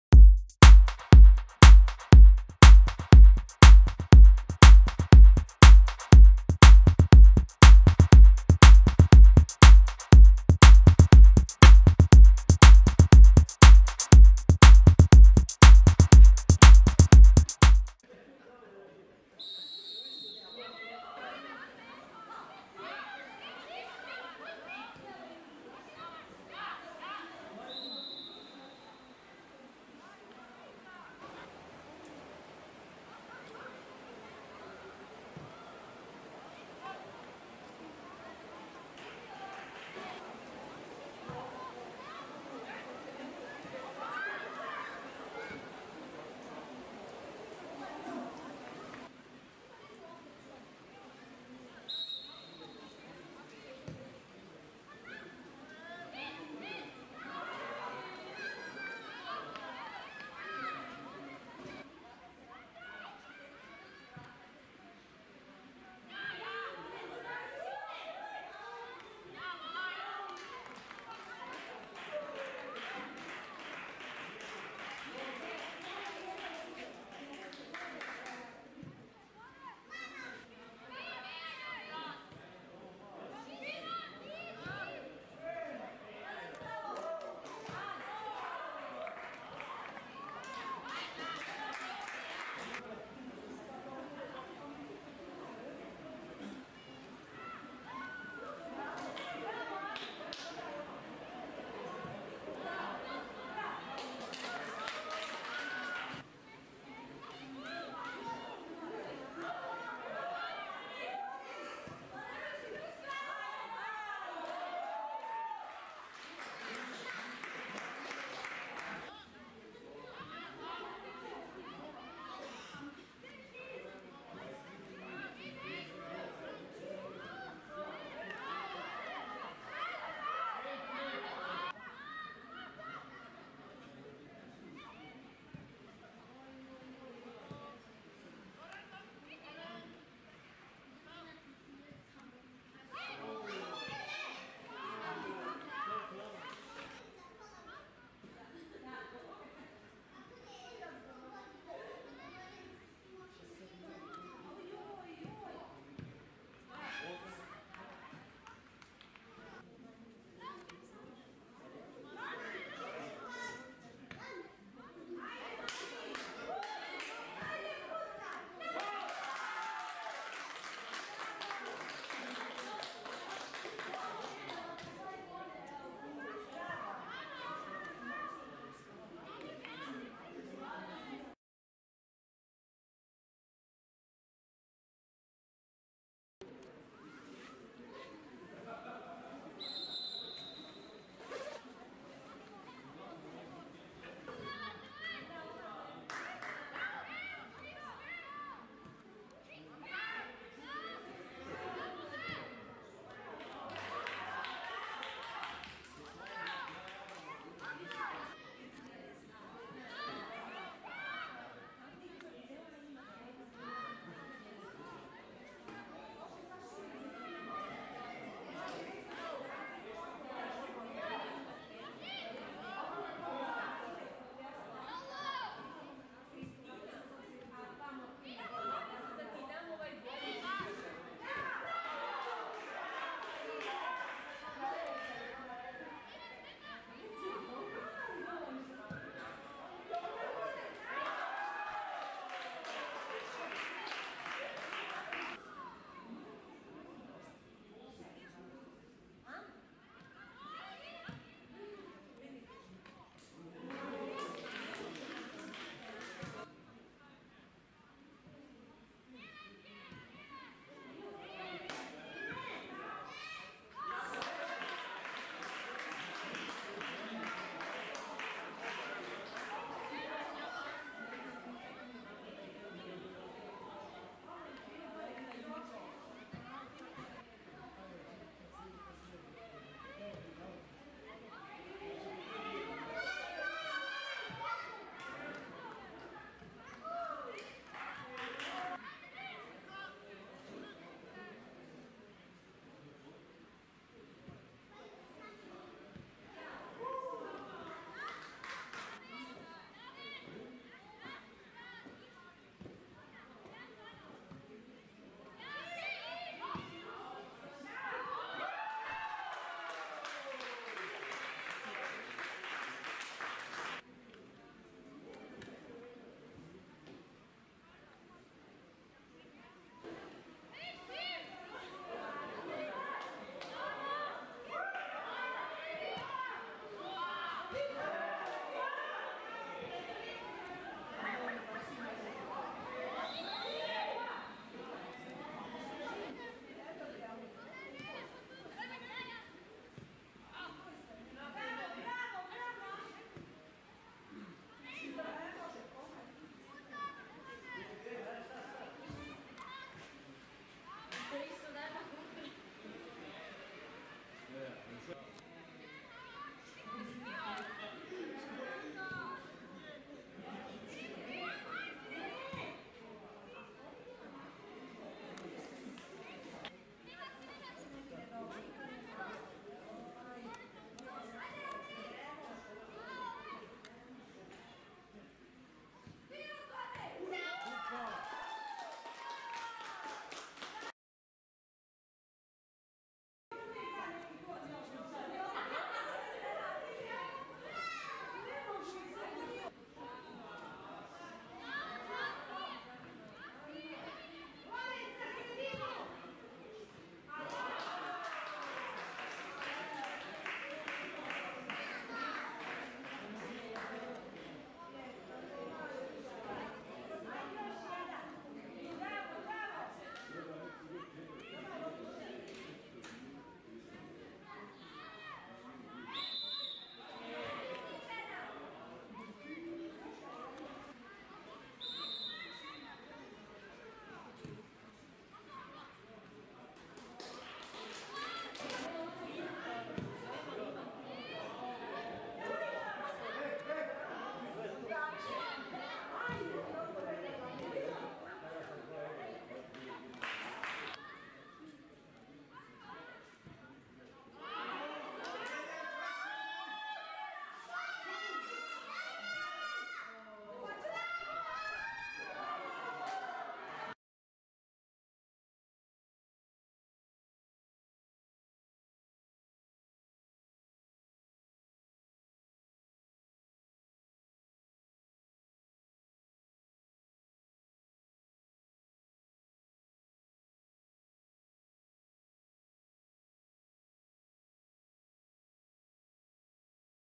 ▶️ U13 - 1. Runde Gebiet Graz ⚽️ ESK Graz 🆚 JSV Mariatrost ⚽️ vom 07.09.2019
⚽️ 1. Runde Gebiet Graz U13 A ⚽️
▶️ Endstand 2:6 (1:2)
▶️ ASKÖ-Stadion Eggenberg
⚽️ Torschützen für den ESK Graz ⚽️
⚽️ Torschützen für JSV Mariatrost ⚽️
KlemensSemlitsch 5x